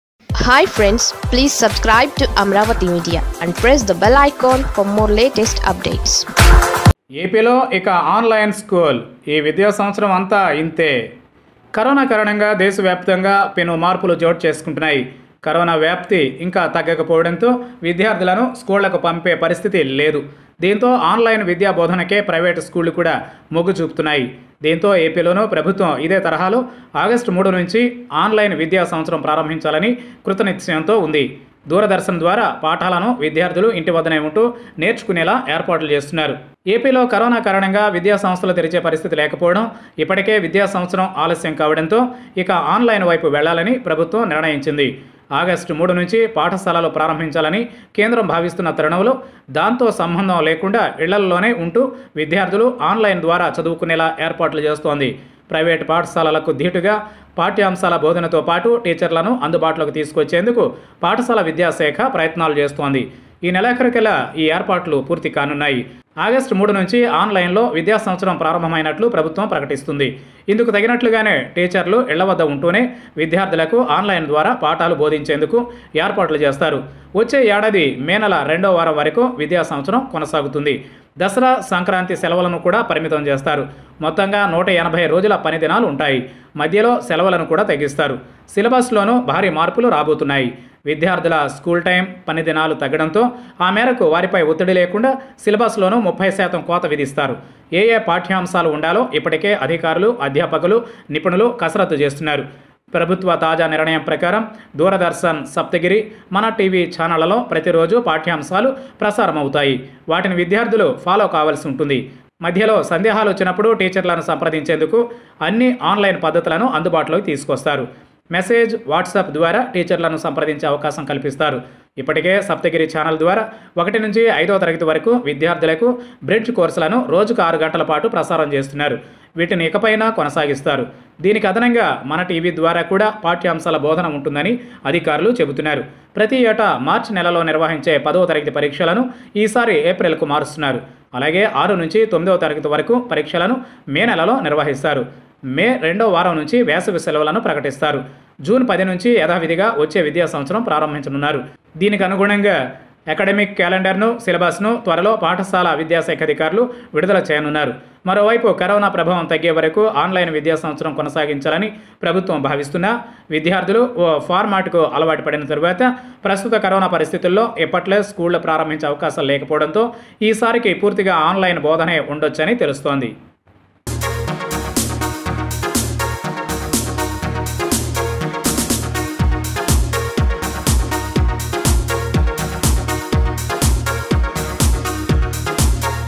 ఏపీలో స్టూడెంట్స్ ఇక స్కూలుకు వెళ్ళక్కర్లేదు..!| AP Govt Plans to Start Online Education in Schools
Andhra Pradesh Government Has Decided to Start Education Year from August 3rd Through Online.Through this, students take lessons on the Doordarshan Saptagiri and Mana TV channels.